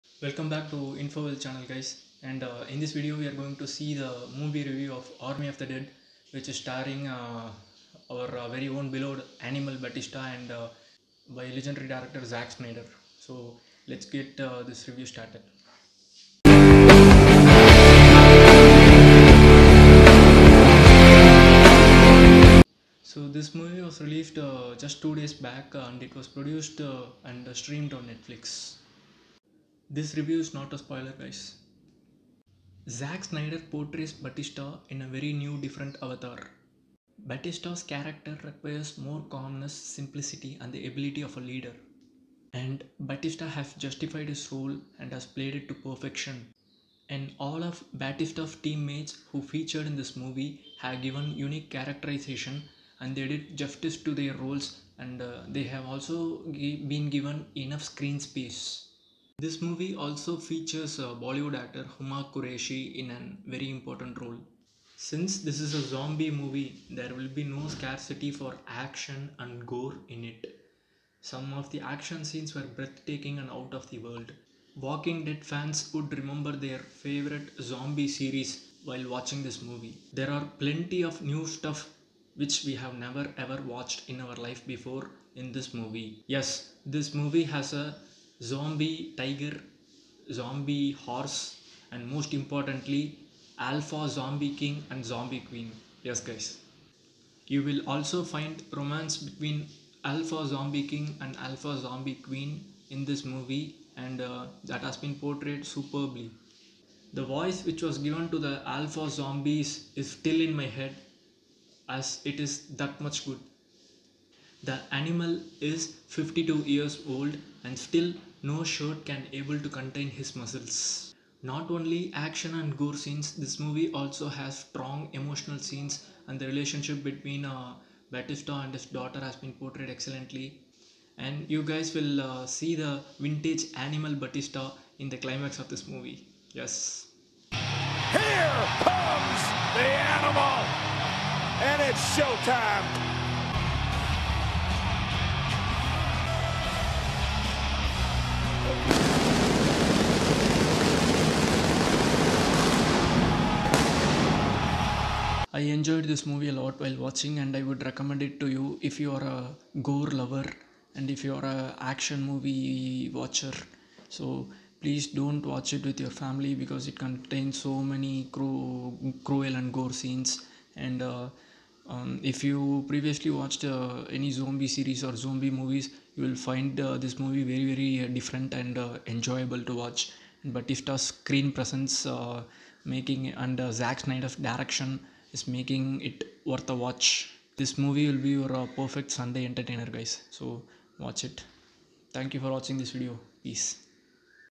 Army of the Dead Movie Review - Animal 'Batista', Zack Snyder - Info Wealth
Vintage Animal Batista + Zack Snyder + Alpha zombies is Carnage 🔥🔥